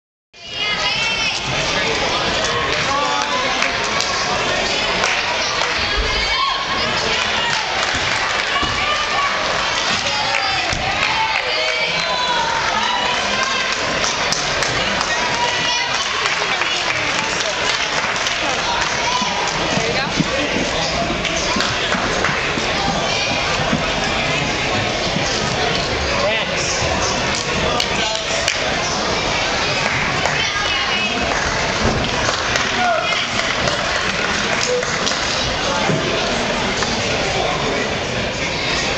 Jack's Vault - 1/27/07
Jack fell on his hinder at the Mardi Gras Invidational.  Yet, he still scored an 8.9.